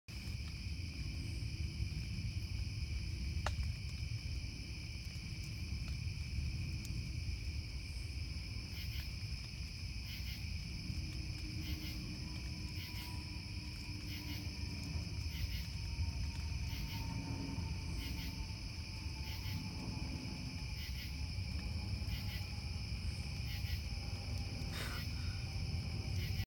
Marbled Salamander (Ambystoma opacum) Interactions on “Big Night”, 8/2019
Observed 9 pairs of male marbled salamanders engaged in the above behavior. Massachusetts.